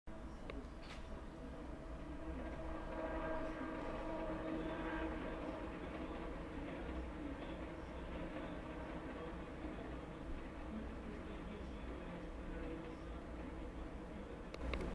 M 48844 {madeira} Tui-Devesas a reboque da LD Euro 4000 6005 Takargo (06.10.2016)
Imagens captadas a partir de minha casa sobre a PS à Av. Sá Carneiro {P. k. 4,000 da linha do Minho} (Rio Tinto - Gondomar)